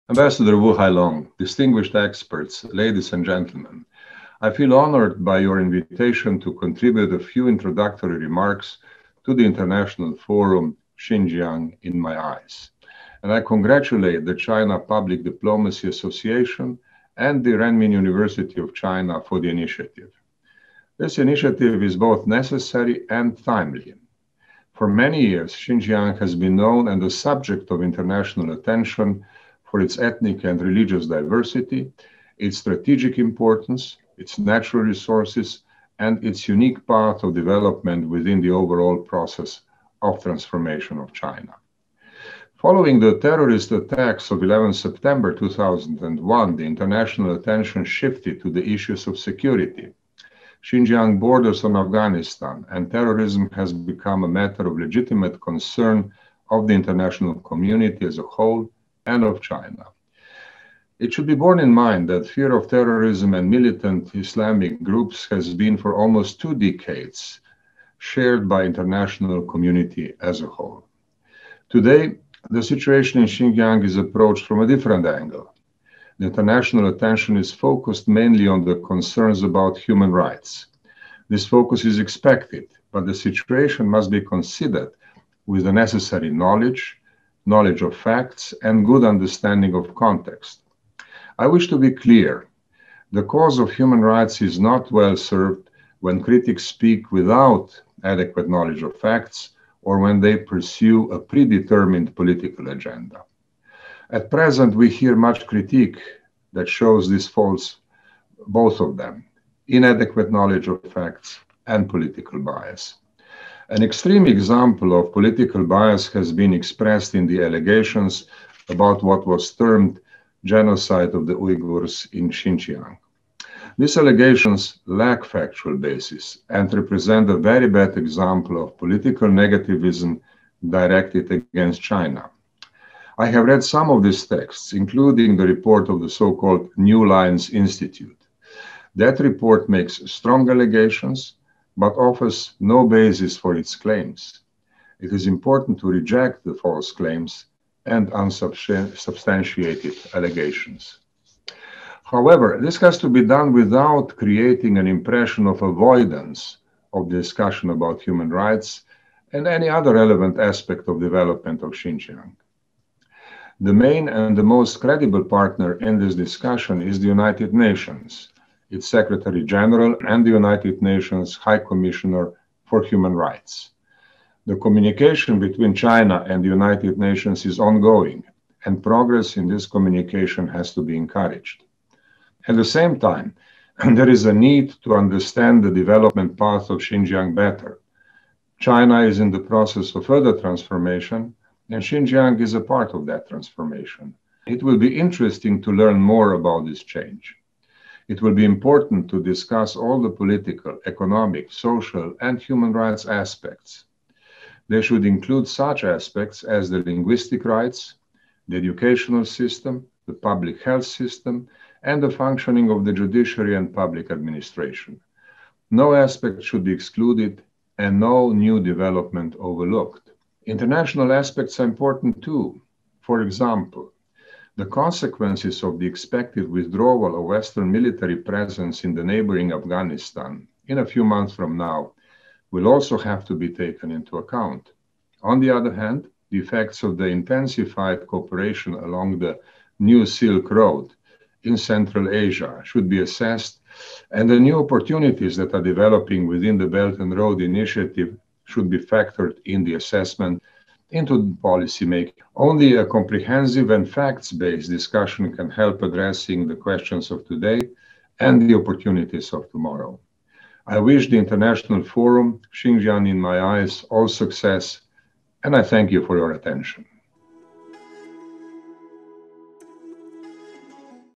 Danilo Turk: Xinjiang human rights is an example of political bias
Former president of Slovenia, Danilo Turk was invited to give a remark on "Xinjiang in my eyes"seminar.  He points out that the cause of human rights is not well served when critics speak without adequate knowledge of facts, and Xinjiang human rights is an example of political bias.